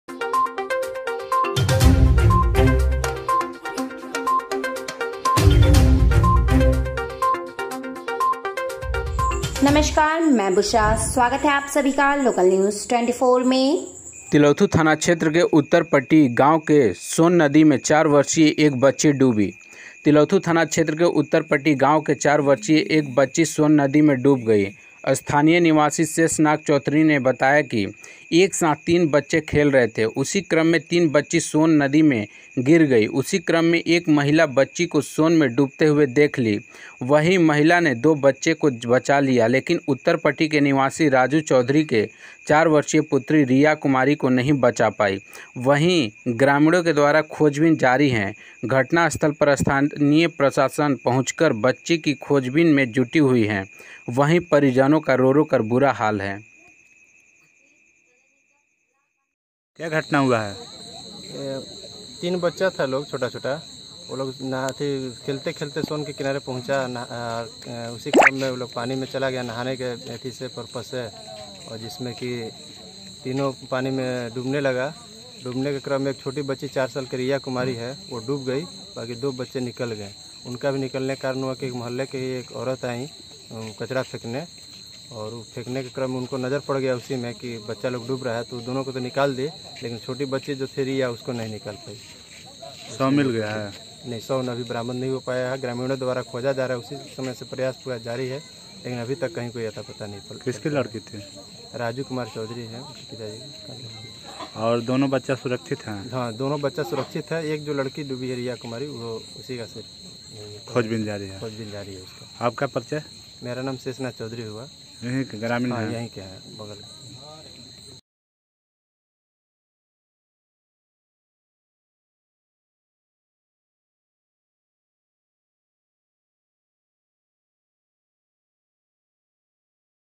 सोन नदी में एक बच्ची डूबी
तिलौथू थाना क्षेत्र के उत्तर पट्टी  गांव के एक बच्ची सोन नदी में डूब गई ।
आपको बता दें कि एक साथ तीन बच्चे खेल रहे थे । उसी क्रम में 3 बच्ची सोन नदी में गिर गई । वही दो बच्चे को एक महिला ने बचा लिया । लेकिन एक बच्ची नहीं बच पाई । वही ग्रामीणों के द्वारा खोजबीन जारी है । घटनास्थल पर स्थानीय प्रशासन पहुंचकर बच्ची की खोजबीन में जुटी हुई है।